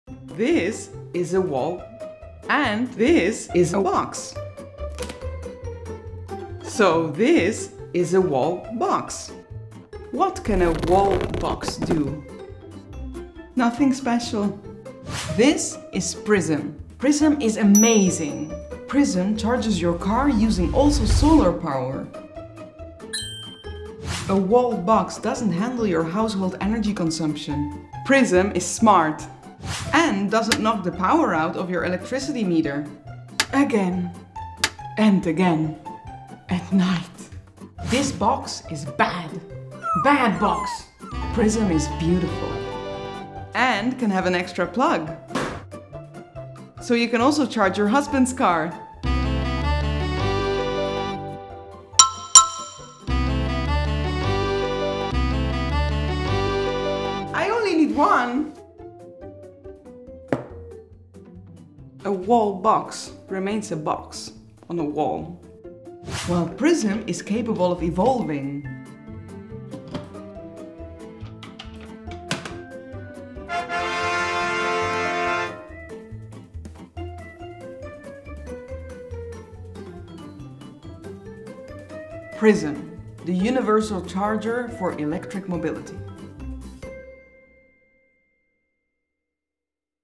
This is a Wall... and this is a Box.
This is a Wall... and this is a Box. Abbiamo una scatola sul muro, niente di che. Poi abbiamo PRISM, il caricatore per la ricarica di veicoli elettrici.

Una wall - box non è smart, non gestisce la potenza installata al tuo contatore, non regola automaticamente la carica erogata all'auto senza mai superare la potenza effettivamente disponibile, ecc. Invece PRISM fa questo e ha tante altre features! Addirittura esiste una versione di PRISM che ti permette di mettere in carica due vetture allo stesso tempo!  E se hai un impianto fotovoltaico, PRISM usa l'energia solare per ricaricare la tua auto.

Caricare un auto elettrica non è mai stato così facile. Trovi PRISM nel nostro shop online a

Se vuoi rimanere informato riguardo a tutte le novità della famiglia Prism ci trovi anche su:

Unisciti a quelli che ormai fanno parte della mobilità sostenibile! Ti forniremo il caricatore per macchine elettriche più adatto alle tue necessità.